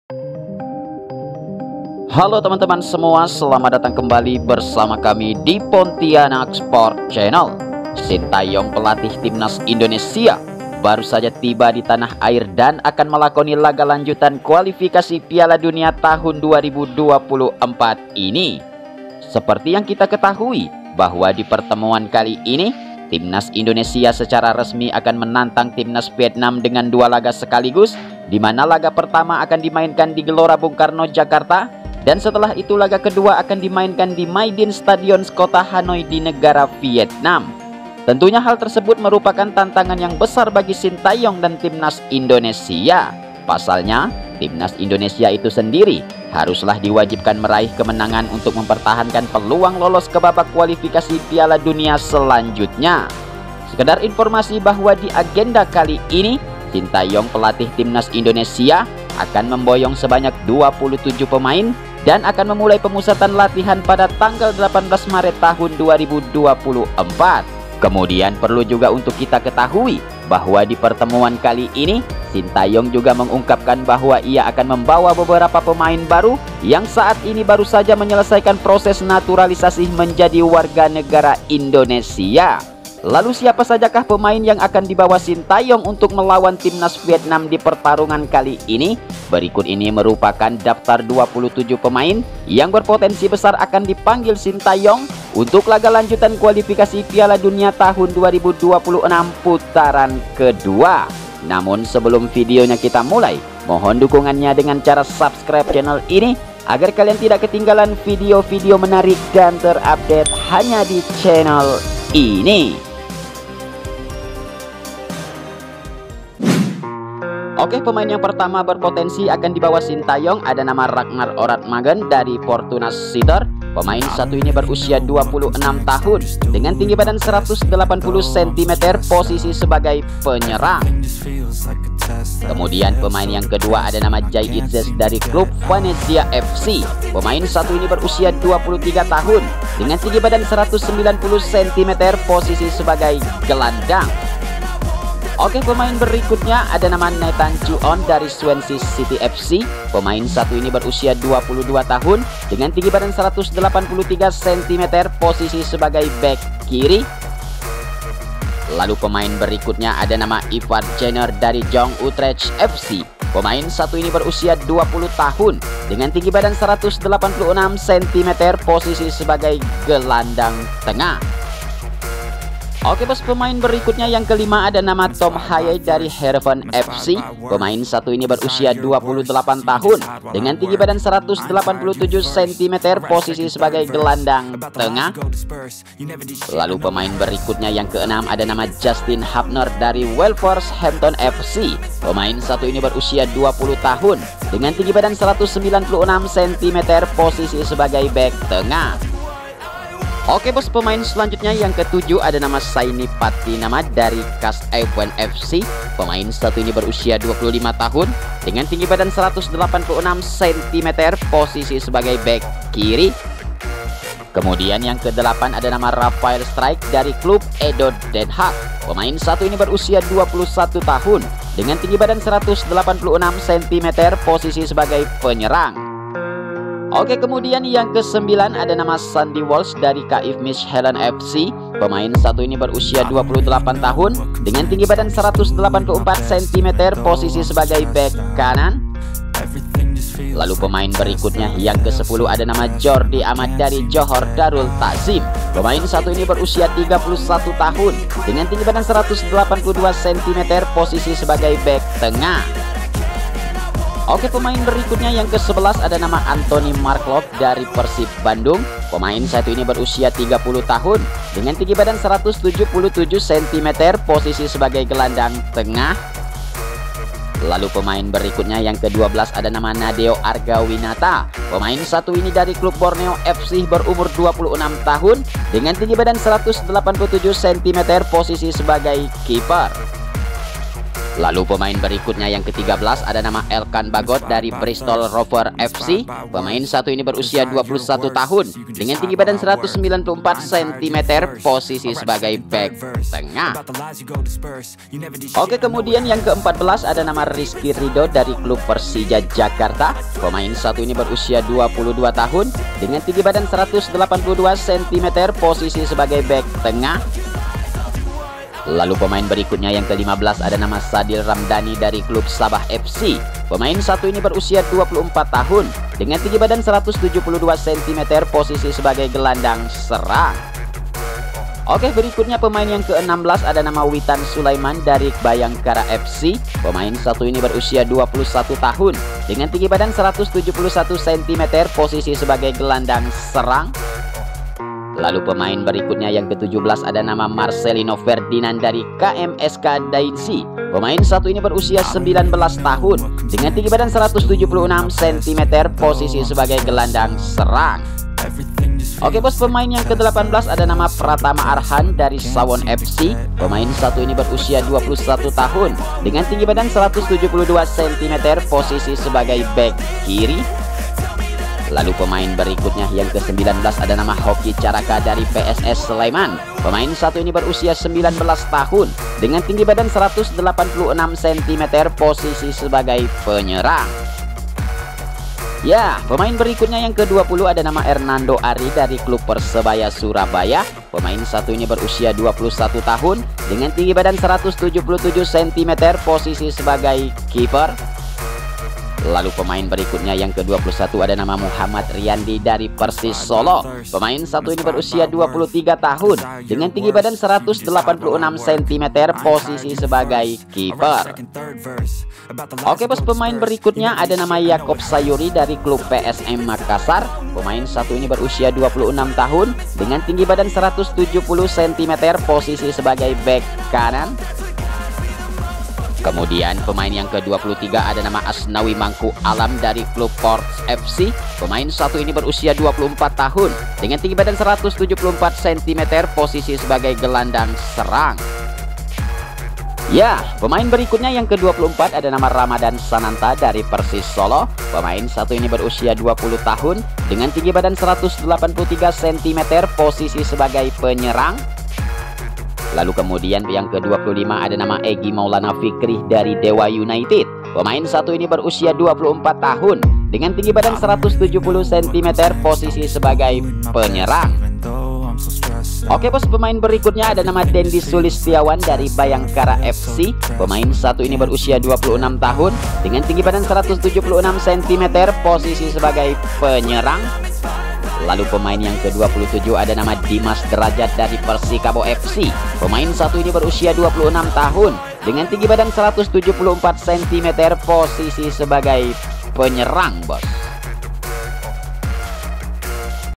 BIKIN VIETNAM MELELEH. DAFTAR 27 PEMAIN TIMNAS INDONESIA SIAP DIPANGIL UNTUK KUALIFIKASI PIALA DUNIA
daftar pemain yg berpotensi besar akan dipanggil sty untuk melawan Vietnam dikualifikasi piala dunia 2026 putaran kedua
daftar 27 pemain untuk hadapi timnas Vietnam pada 21 Maret 2024 laga lanjuta kualifikasi piala dunia 2026
sty akan panggil 27 pemain untuk hadapi Vietnam 2024